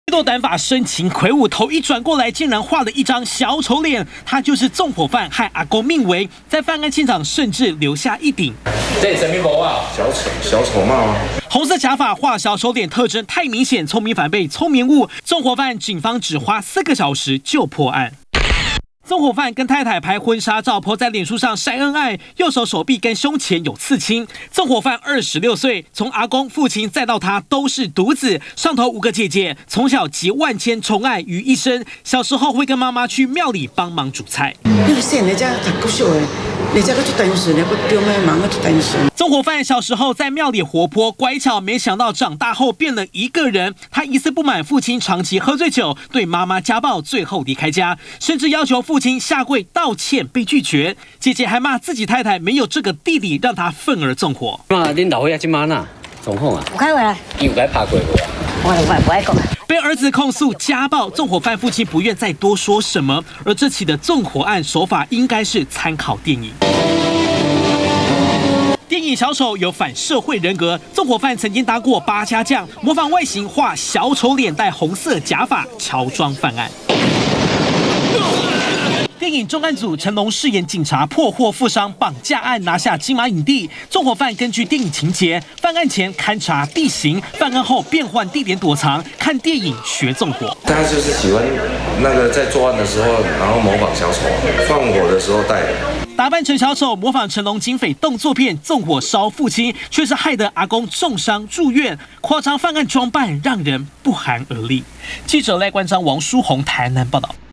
20200802中天新聞 悚！ 爸爸家暴媽媽 子畫「小丑妝」 學電影縱火報復
這名縱火犯，有5個姊姊，他是家中唯一男孩，可以說是從出生，就集萬千寵愛於一身，但不滿父親長期酒後家暴媽媽，曾經要求父親到廟裡下跪道歉被拒絕，種種原因，讓他憤而縱火，平時喜歡看電影，因為曾經當過八家將，自畫小丑妝，喬裝犯案，也根據成龍警匪動作片，重案組，規劃犯案過程以及，逃跑方式。